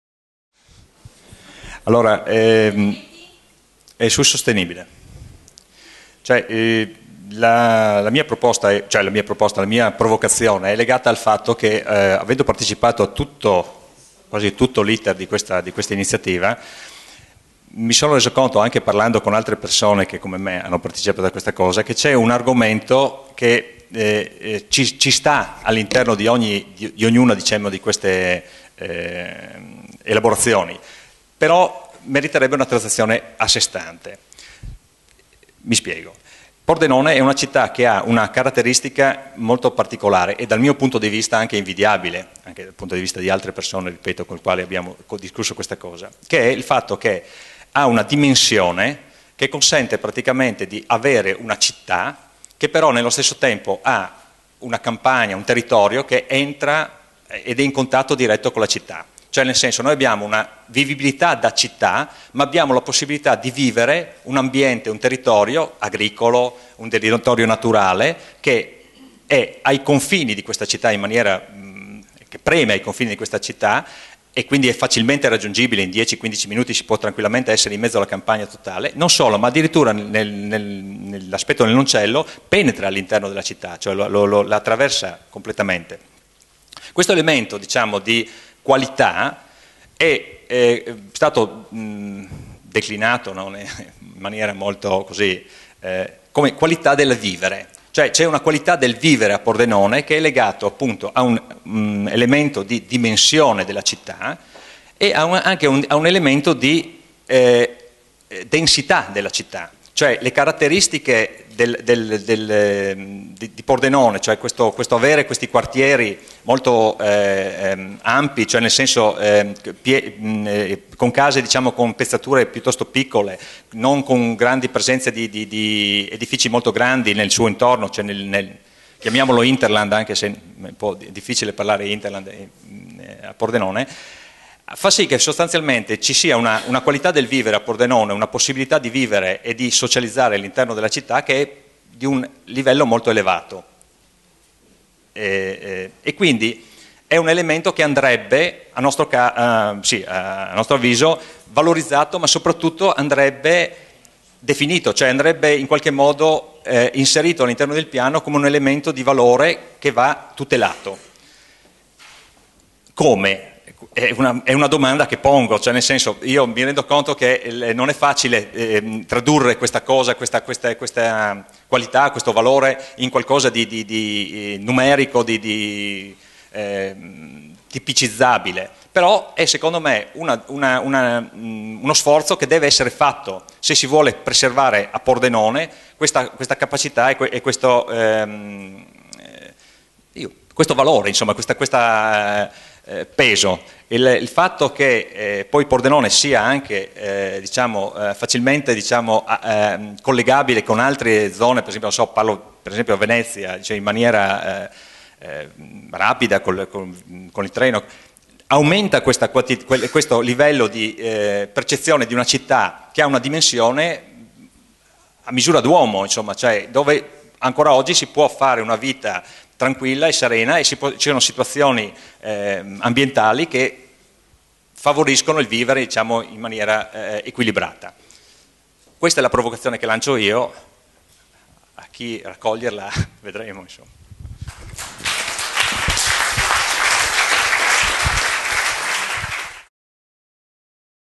La ricerca della qualità, Walter Coletto [Pordenone più facile]
"Il Piano come occasione di valorizzazione e ricerca della qualità": intervento di Walter Coletto al simposio conclusivo della prima parte di Pordenone più facile (Palazzo Badini, 15 dicembre 2012), il percorso partecipato verso il nuovo Piano regolatore generale della città